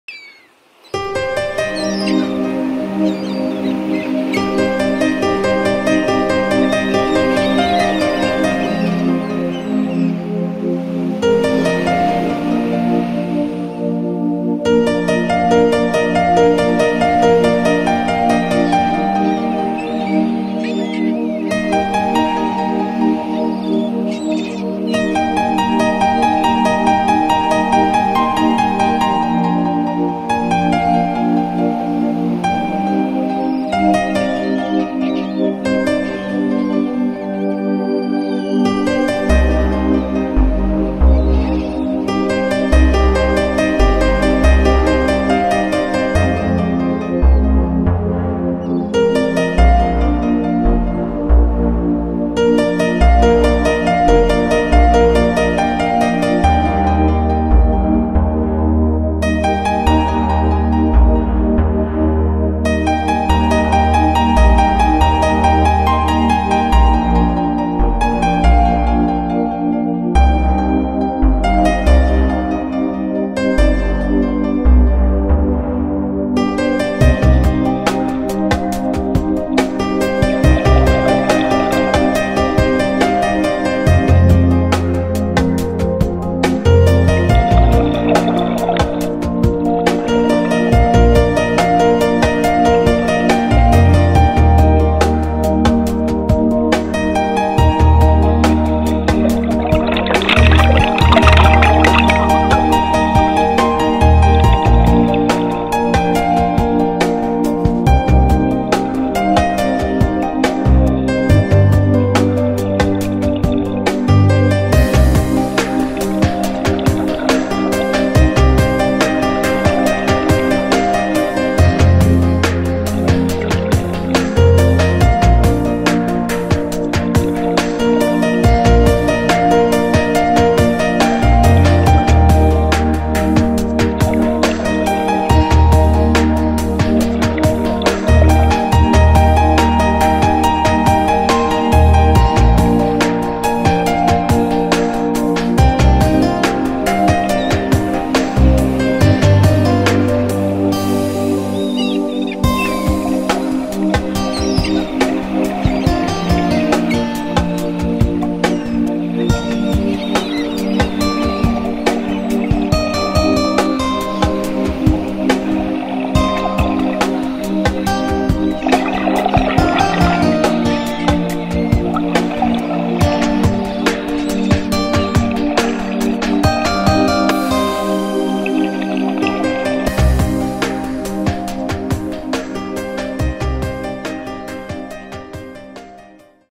Flavour of LANZAROTE in a WEEK - DAY SIX
DAY SIX - Diving in the area around of Puerto del Carmen